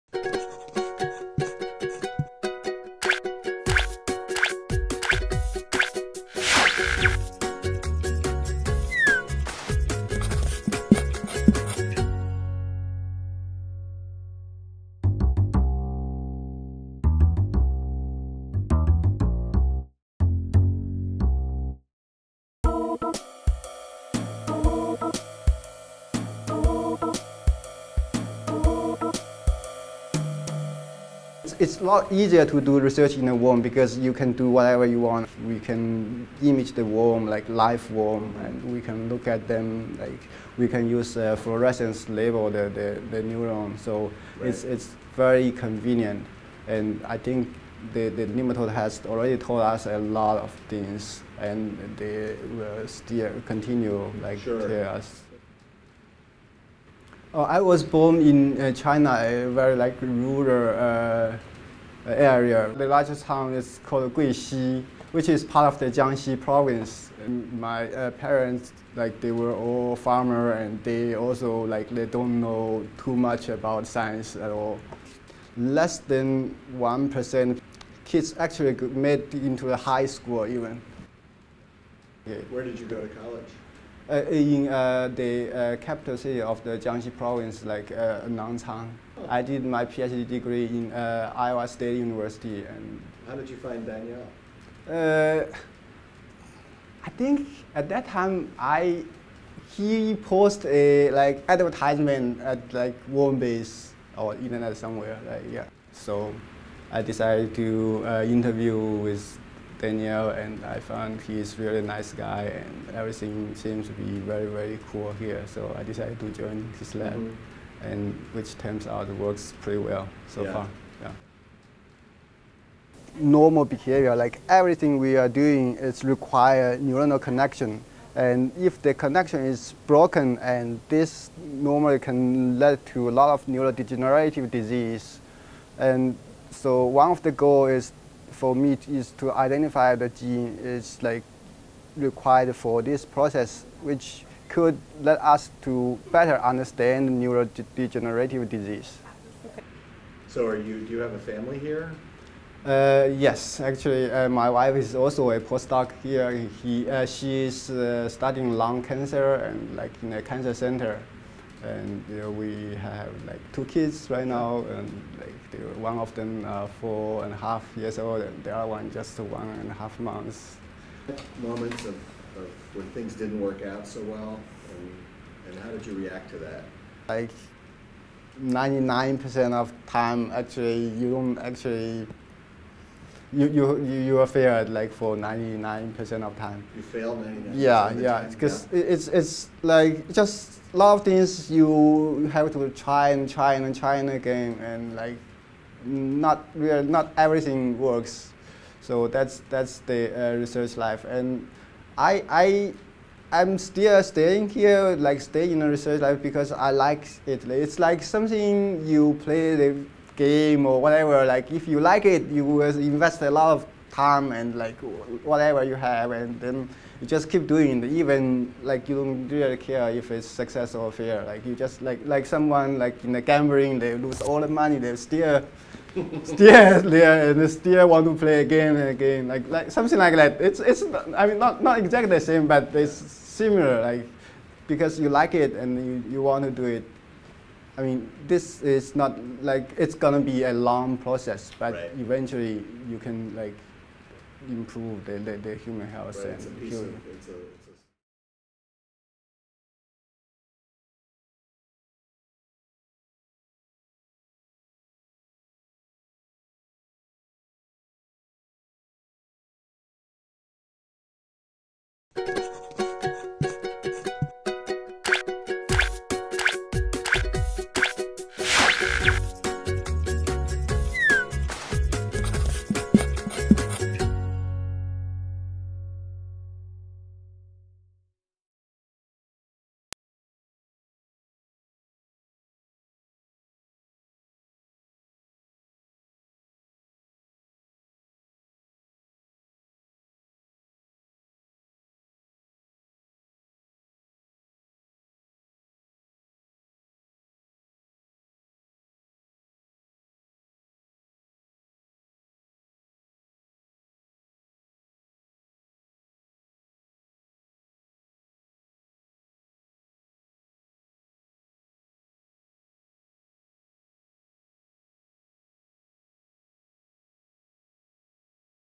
"You Don't Care If It's a Success or Failure" Says Zhiyong
This LabTV role model video was produced by Yale University's media department, and won an award for an overall outstanding collection of role model videos. Meet Zhiyong, a post-doc candidate at Yale University in the Colon-Ramos Lab. He studies neuroscience and is curious about broken connections in the brain. His research contribute to learning more about depression and other neurodegenerative diseases.